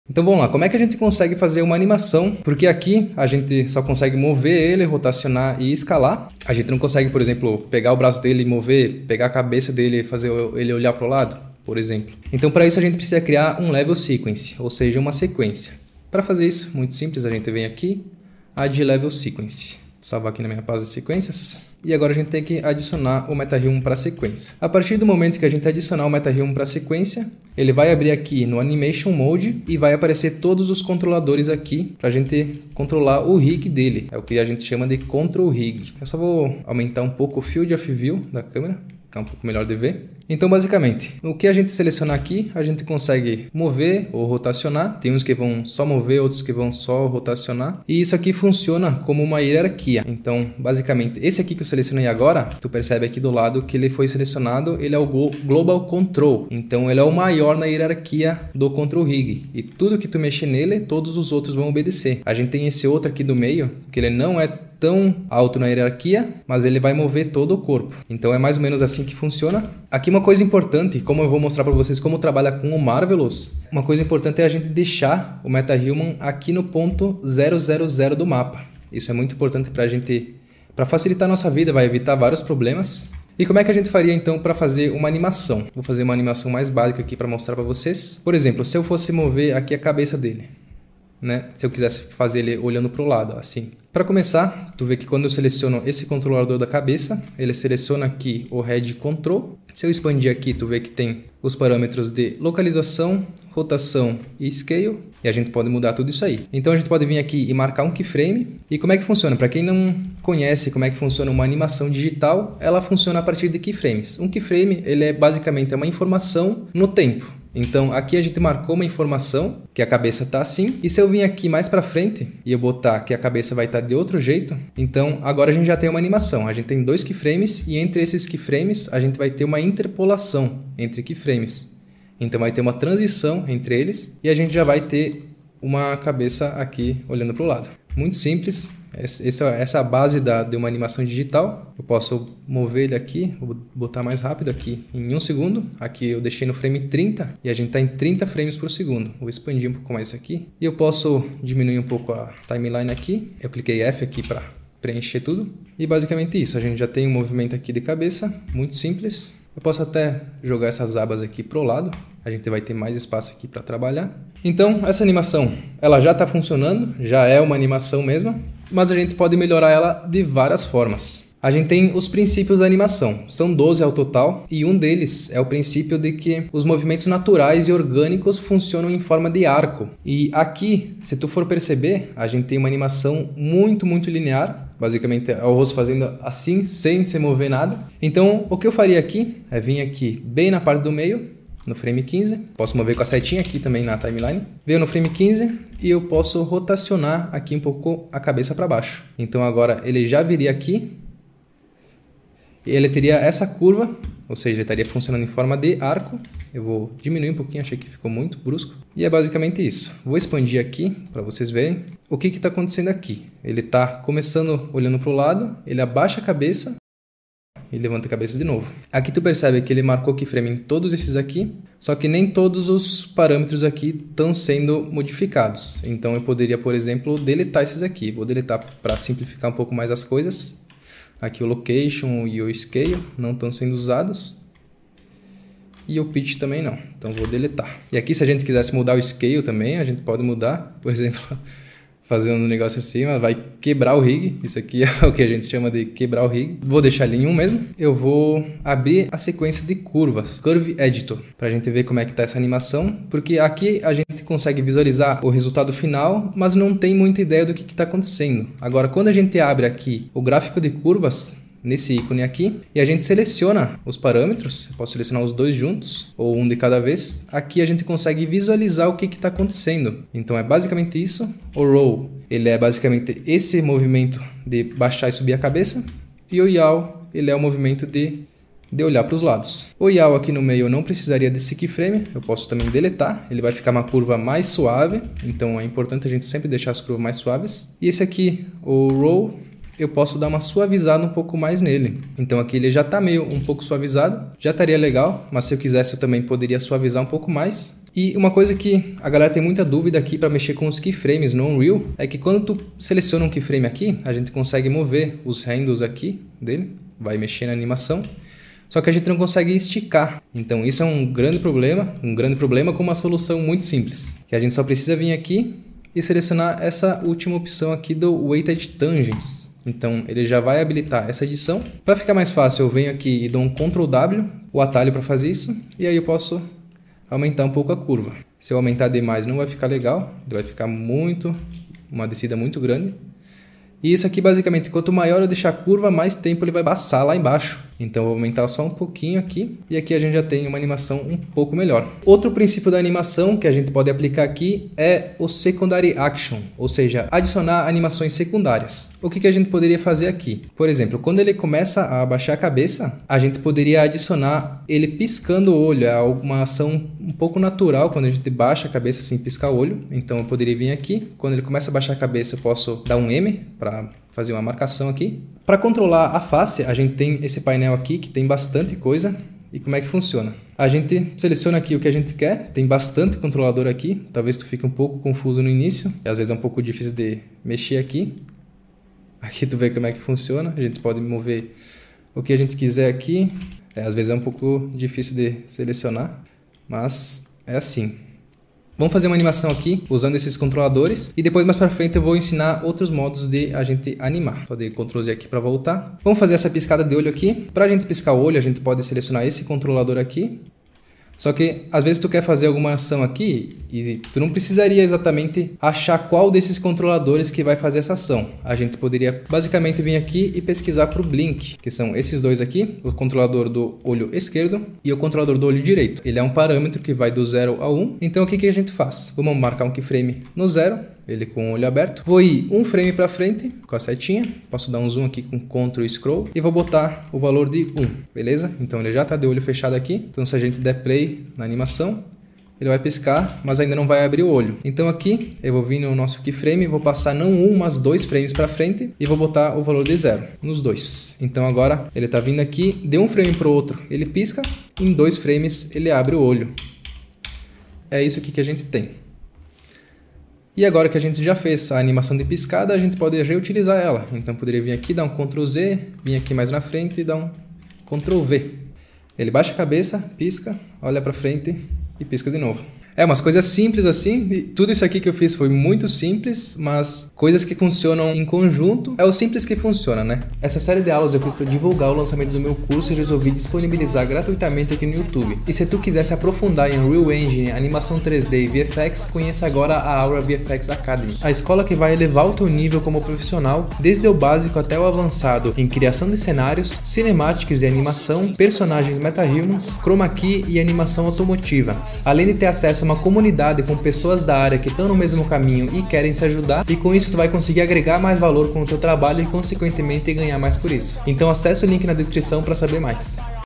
Aprenda a Animar MetaHumans no Unreal Engine 5: Tutorial Iniciantes
Tutorial para iniciantes sobre como animar personagens MetaHumans no Unreal Engine 5 usando Control Rig! 🌟

Neste tutorial você aprenderá os fundamentos para começar a criar animações incríveis com MetaHumans. Não importa se você é novo no mundo da animação ou no Unreal Engine, este tutorial vai te guiar passo a passo.

🔹 O que você vai aprender:
1. Introdução aos MetaHumans e Control Rig
2. Técnicas de animação usando o Sequencer
3. Dicas e truques de animação para iniciantes

Me siga no Instagram para mais dicas, tutoriais e novidades exclusivas!